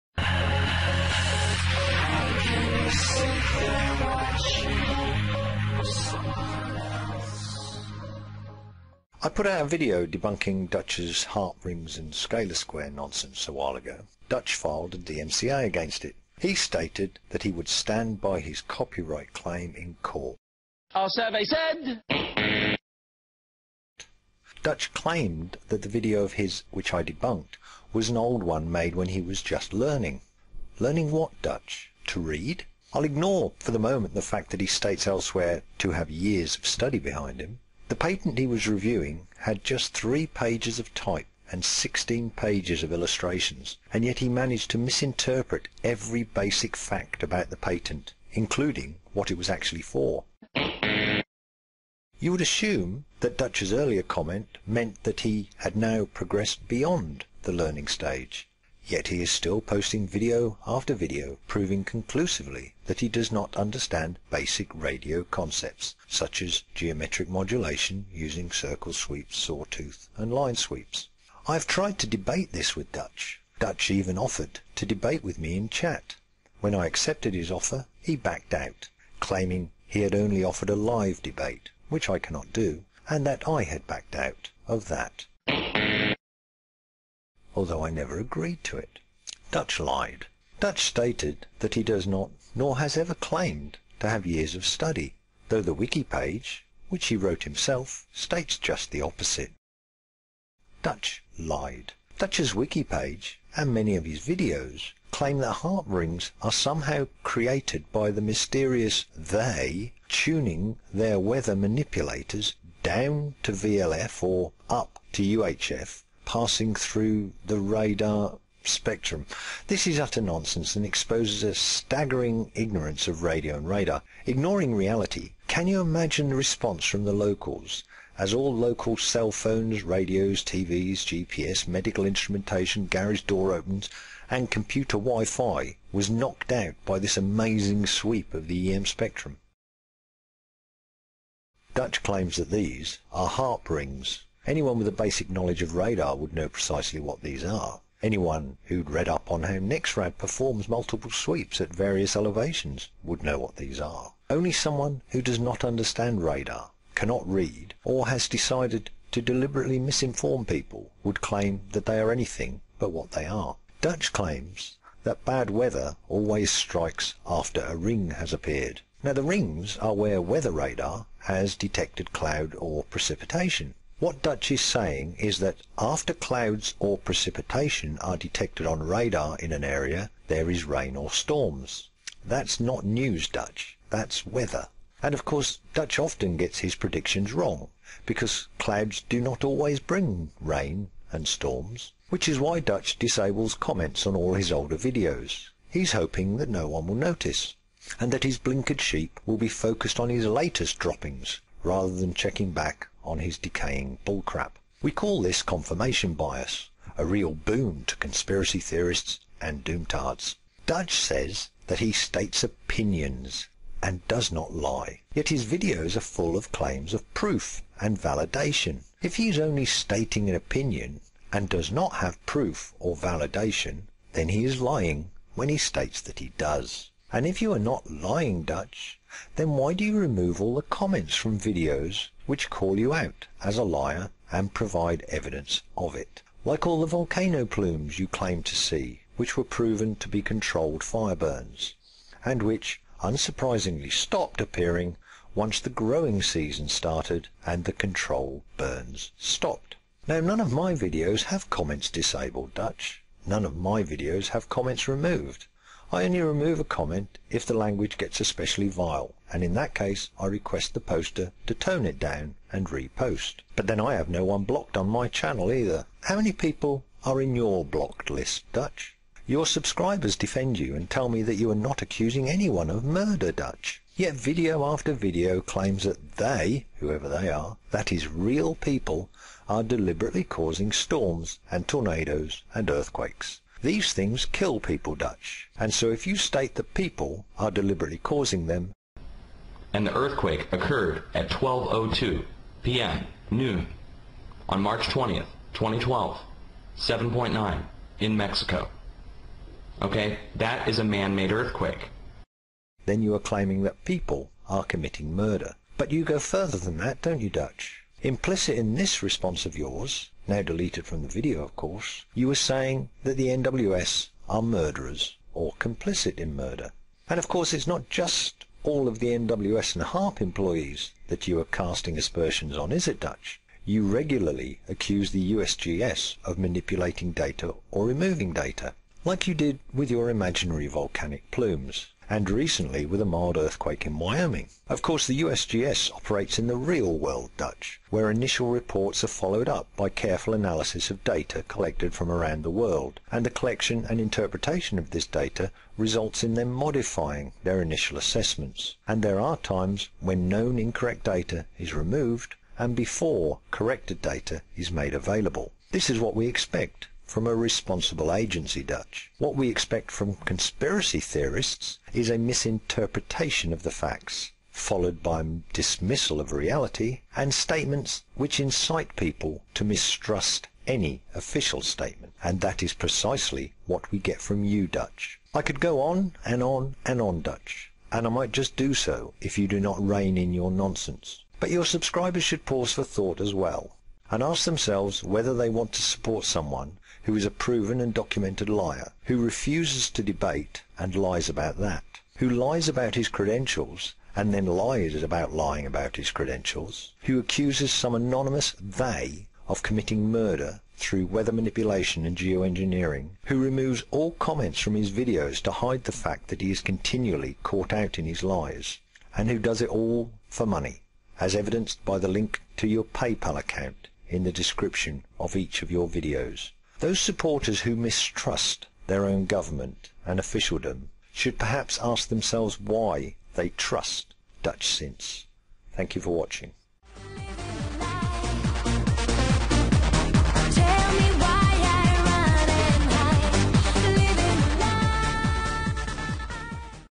Dutchsinse Epic Fails - mirror
Published on May 9, 2012 by Huttate1
If you agree with this please hit the Remix This Video Button.
The more channels this is on the better - his followers need educating.

Google nexrad hoax history to see how old this fail is (Thanks dutchsencee)

Now that Dutch's DMCA has been proven false. I take a look at him one more time.

There are a few channels still trying to counter Dutch's conspiracy BS and educate his subscribers. It cannot be done on Dutch's channels as he blocks all detractors.